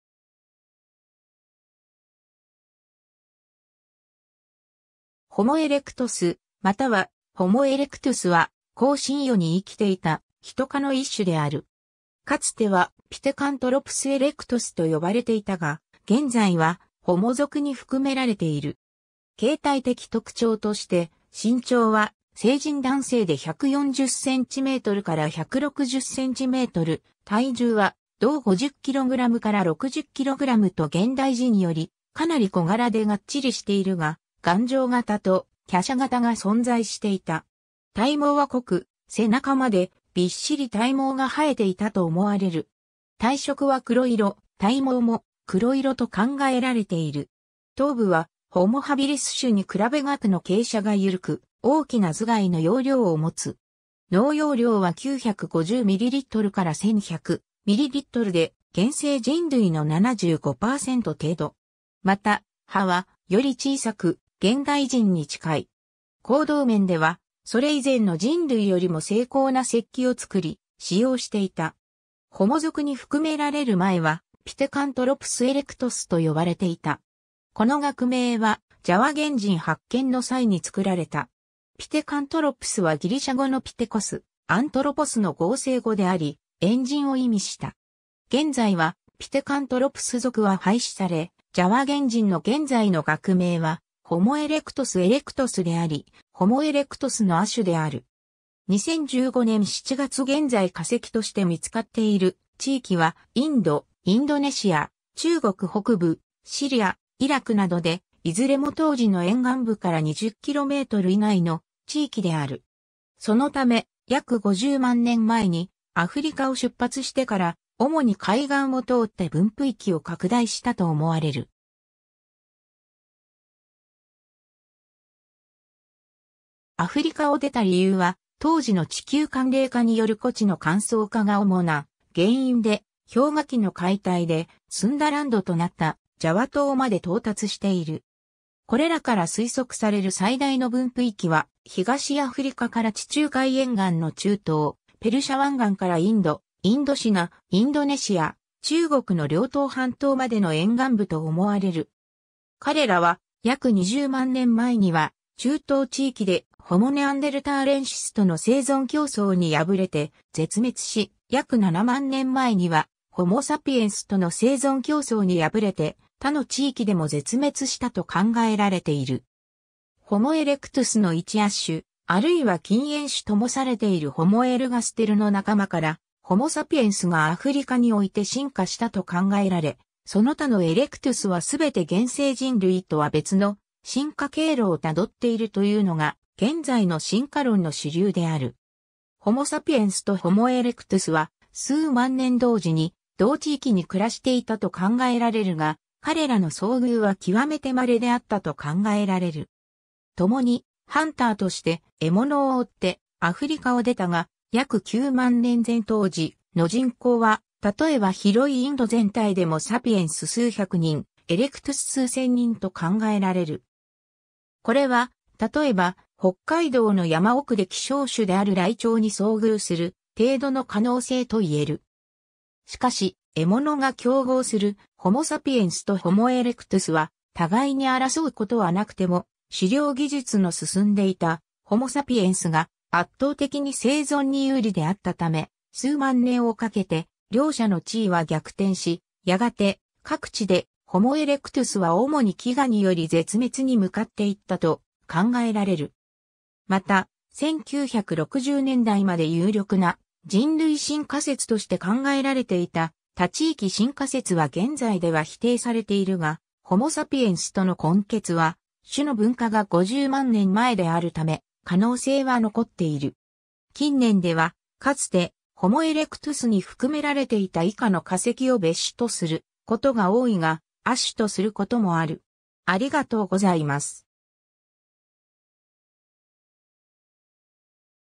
ホモ・エレクトス
ホモ・エレクトス（Homo erectus）またはホモ・エレクトゥスは、更新世に生きていたヒト科の一種である。
かつてはピテカントロプス・エレクトスと呼ばれていたが、現在はホモ属（ヒト属）に含められている。
 形態的特徴として、身長は成人男性で140cm～160cm、体重は同50kg～60kgと現代人よりかなり小柄でがっちりしているが、頑丈型と華奢型が存在していた。
体毛は濃く、背中までびっしり体毛が生えていたと思われる。
体色は黒色、体毛も黒色と考えられている。
 頭部はホモ・ハビリス種に比べ額の傾斜がゆるく、大きな頭蓋の容量を持つ。
脳容量は950ミリリットルから1100ミリリットルで、現生人類の75%程度。
また、歯はより小さく、現代人に近い。
 行動面では、それ以前の人類よりも精巧な石器を作り、使用していた。
 ホモ属に含められる前はピテカントロプス・エレクトス（Pithecanthropus erectus）と呼ばれていた。
この学名はジャワ原人発見の際に作られた。
ピテカントロプスはギリシャ語のピテコス（pitekos 猿）、アントロポス（anthropos 人類）の合成語であり、猿人を意味した。
現在はピテカントロプス属は廃止され、ジャワ原人の現在の学名はホモ・エレクトス・エレクトス（Homo erectus erectus）であり、ホモ・エレクトスの亜種である。
 2015年7月現在化石として見つかっている地域はインド、インドネシア、中国北部、シリア、イラクなどで、いずれも当時の沿岸部から20km以内の地域である。
そのため、約50万年前にアフリカを出発してから主に海岸を通って分布域を拡大したと思われる。
 アフリカを出た理由は当時の地球寒冷化による故地の乾燥化が主な原因で、氷河期の海退でスンダランドとなったジャワ島まで到達している。
これらから推測される最大の分布域は東アフリカから地中海沿岸の中東、ペルシャ湾岸からインド、インドシナ、インドネシア、中国の遼東半島までの沿岸部と思われる。
 彼らは約20万年前には中東地域でホモ・ネアンデルターレンシス（ネアンデルタール人）との生存競争に敗れて絶滅し、約7万年前にはホモ・サピエンスとの生存競争に敗れて他の地域でも絶滅したと考えられている。
 ホモ・エレクトゥスの1亜種、あるいは近縁種ともされているホモ・エルガステルの仲間からホモ・サピエンスがアフリカにおいて進化したと考えられ、その他のエレクトゥスは全て現生人類とは別の進化経路を辿っているというのが現在の進化論の主流である。
 ホモ・サピエンスとホモ・エレクトゥスは数万年同時に同地域に暮らしていたと考えられるが、彼らの遭遇は極めてまれであったと考えられる。
ともにハンターとして獲物を追ってアフリカを出たが、約9万年前当時の人口は、たとえば広いインド全体でもサピエンス数百人、エレクトゥス数千人と考えられる。
（この数値は公式な発表では存在せず、あくまでも数人の研究者の推測である為、時代により相当な誤差が生じる可能性は否定できない）。
これは、たとえば北海道の山奥で希少種である雷鳥に遭遇する程度の可能性と言える。
しかし、獲物が競合するホモ・サピエンスとホモ・エレクトゥスは、互いに争うことはなくても、狩猟技術の進んでいたホモ・サピエンスが圧倒的に生存に有利であったため、数万年をかけて両者の地位は逆転し、やがて、各地でホモ・エレクトゥスは主に飢餓により絶滅に向かっていったと考えられる。
 また、1960年代まで有力な人類進化説として考えられていた多地域進化説は現在では否定されているが、ホモ・サピエンスとの混血は、種の分化が50万年前であるため、可能性は残っている。
 近年では、かつてホモ・エレクトゥスに含められていた以下の化石を別種とすることが多いが、亜種とすることもある。